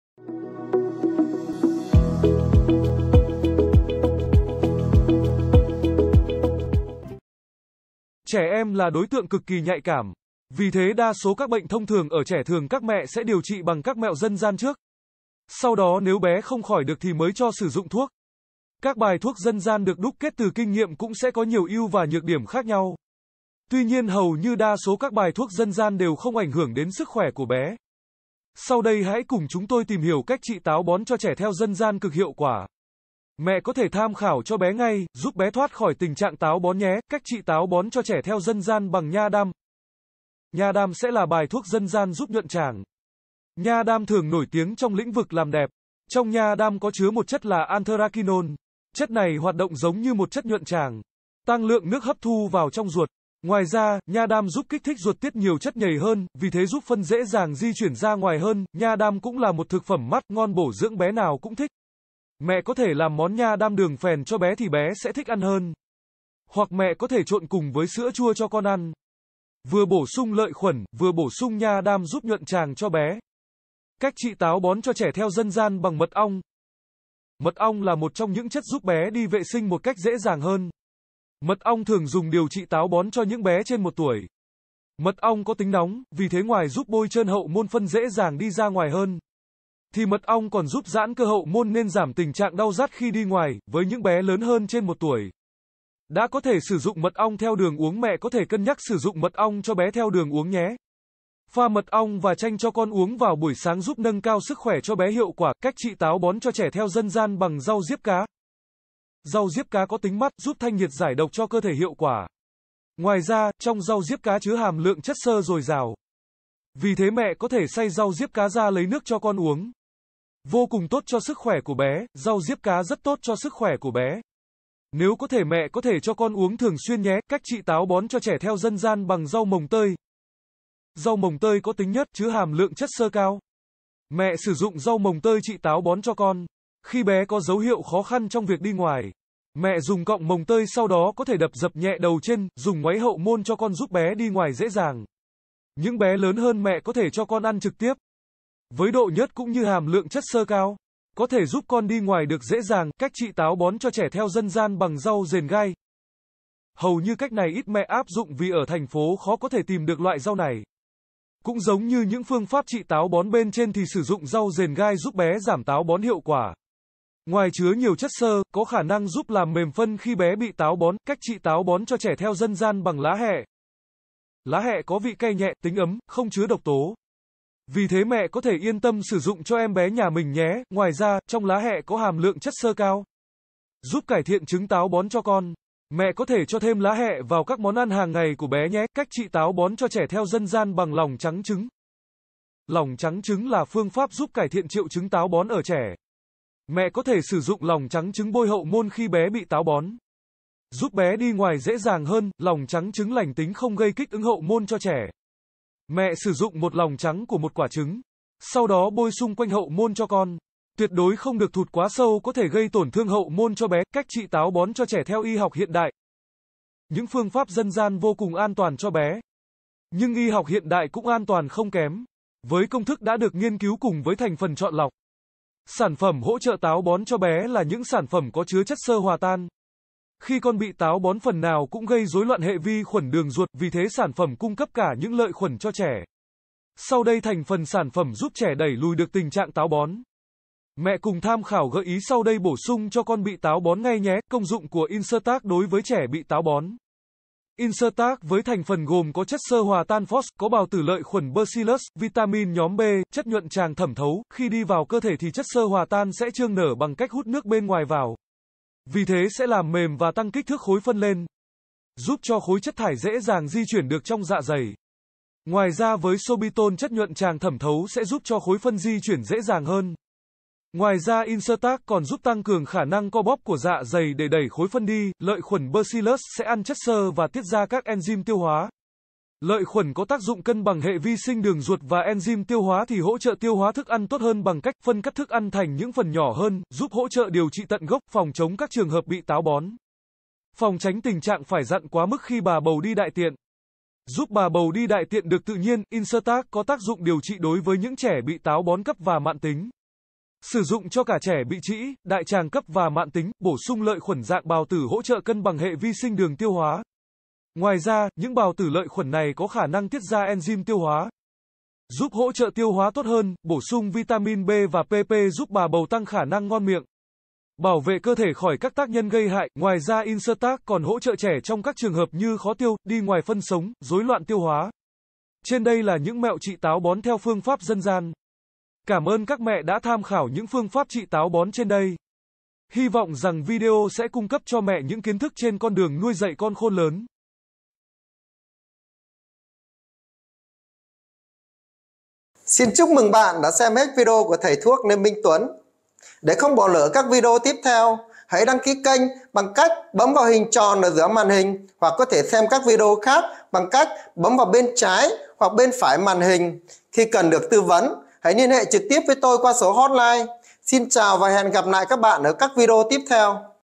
MẸO DÂN GIAN CHỮA TÁO BÓN CHO TRẺ CỰC AN TOÀN VÀ HIỆU QUẢ
Trẻ em là đối tượng cực kỳ nhạy cảm. Vì thế đa số các bệnh thông thường ở trẻ thường các mẹ sẽ điều trị bằng các mẹo dân gian trước. Sau đó nếu bé không khỏi được thì mới cho sử dụng thuốc. Các bài thuốc dân gian được đúc kết từ kinh nghiệm cũng sẽ có nhiều ưu và nhược điểm khác nhau. Tuy nhiên hầu như đa số các bài thuốc dân gian đều không ảnh hưởng đến sức khỏe của bé. Sau đây hãy cùng chúng tôi tìm hiểu cách trị táo bón cho trẻ theo dân gian cực hiệu quả. Mẹ có thể tham khảo cho bé ngay giúp bé thoát khỏi tình trạng táo bón nhé.

Cách trị táo bón cho trẻ theo dân gian bằng nha đam
Nha đam sẽ là bài thuốc dân gian giúp nhuận tràng. Nha đam thường nổi tiếng trong lĩnh vực làm đẹp. Trong nha đam có chứa 1 chất là anthraquinone. Chất này hoạt động giống như một chất nhuận tràng, tăng lượng nước hấp thu vào trong ruột. Ngoài ra, nha đam giúp kích thích ruột tiết nhiều chất nhầy hơn, vì thế giúp phân dễ dàng di chuyển ra ngoài hơn.

Nha đam cũng là một thực phẩm mát, ngon bổ dưỡng bé nào cũng thích. Mẹ có thể làm món nha đam đường phèn cho bé thì bé sẽ thích ăn hơn. Hoặc mẹ có thể trộn cùng với sữa chua cho con ăn. Vừa bổ sung lợi khuẩn, vừa bổ sung nha đam giúp nhuận tràng cho bé.

Cách trị táo bón cho trẻ theo dân gian bằng mật ong
Mật ong là một trong những chất giúp bé đi vệ sinh một cách dễ dàng hơn. Mật ong thường dùng điều trị táo bón cho những bé trên 1 tuổi. Mật ong có tính nóng, vì thế ngoài giúp bôi trơn hậu môn phân dễ dàng đi ra ngoài hơn. Thì mật ong còn giúp giãn cơ hậu môn nên giảm tình trạng đau rát khi đi ngoài.

Với những bé lớn hơn trên 1 tuổi. Đã có thể sử dụng mật ong theo đường uống mẹ có thể cân nhắc sử dụng mật ong cho bé theo đường uống nhé. Pha mật ong và chanh cho con uống vào buổi sáng giúp nâng cao sức khỏe cho bé hiệu quả.

Cách trị táo bón cho trẻ theo dân gian bằng rau diếp cá
Rau diếp cá có tính mát, giúp thanh nhiệt giải độc cho cơ thể hiệu quả. Ngoài ra, trong rau diếp cá chứa hàm lượng chất xơ dồi dào.

Vì thế mẹ có thể xay rau diếp cá ra lấy nước cho con uống. Vô cùng tốt cho sức khỏe của bé.
Rau diếp cá rất tốt cho sức khỏe của bé. Nếu có thể mẹ có thể cho con uống thường xuyên nhé.

Cách trị táo bón cho trẻ theo dân gian bằng rau mồng tơi
Rau mồng tơi có tính nhớt, chứa hàm lượng chất xơ cao. Mẹ sử dụng rau mồng tơi trị táo bón cho con. Khi bé có dấu hiệu khó khăn trong việc đi ngoài. Mẹ dùng cọng mồng tơi sau đó có thể đập dập nhẹ đầu trên.

Dùng ngoáy hậu môn cho con giúp bé đi ngoài dễ dàng. Những bé lớn hơn mẹ có thể cho con ăn trực tiếp. Với độ nhớt cũng như hàm lượng chất xơ cao. Có thể giúp con đi ngoài được dễ dàng.

Cách trị táo bón cho trẻ theo dân gian bằng rau dền gai
Hầu như cách này ít mẹ áp dụng vì ở thành phố khó có thể tìm được loại rau này. Cũng giống như những phương pháp trị táo bón bên trên thì sử dụng rau dền gai giúp bé giảm táo bón hiệu quả. Ngoài chứa nhiều chất xơ, có khả năng giúp làm mềm phân khi bé bị táo bón.

Cách trị táo bón cho trẻ theo dân gian bằng lá hẹ
Lá hẹ có vị cay nhẹ, tính ấm, không chứa độc tố. Vì thế mẹ có thể yên tâm sử dụng cho em bé nhà mình nhé.
Ngoài ra, trong lá hẹ có hàm lượng chất xơ cao. Giúp cải thiện chứng táo bón cho con. Mẹ có thể cho thêm lá hẹ vào các món ăn hàng ngày của bé nhé.

 Cách trị táo bón cho trẻ theo dân gian bằng lòng trắng trứng
Lòng trắng trứng là phương pháp giúp cải thiện triệu chứng táo bón ở trẻ. Mẹ có thể sử dụng lòng trắng trứng bôi hậu môn khi bé bị táo bón. Giúp bé đi ngoài dễ dàng hơn.

 Lòng trắng trứng lành tính không gây kích ứng hậu môn cho trẻ. Mẹ sử dụng 1 lòng trắng của 1 quả trứng. Sau đó bôi xung quanh hậu môn cho con. Tuyệt đối không được thụt quá sâu có thể gây tổn thương hậu môn cho bé.

Cách trị táo bón cho trẻ theo y học hiện đại
Những phương pháp dân gian vô cùng an toàn cho bé. Nhưng y học hiện đại cũng an toàn không kém. Với công thức đã được nghiên cứu cùng với thành phần chọn lọc. Sản phẩm hỗ trợ táo bón cho bé là những sản phẩm có chứa chất xơ hòa tan. Khi con bị táo bón phần nào cũng gây rối loạn hệ vi khuẩn đường ruột, vì thế sản phẩm cung cấp cả những lợi khuẩn cho trẻ. Sau đây thành phần sản phẩm giúp trẻ đẩy lùi được tình trạng táo bón. Mẹ cùng tham khảo gợi ý sau đây bổ sung cho con bị táo bón ngay nhé.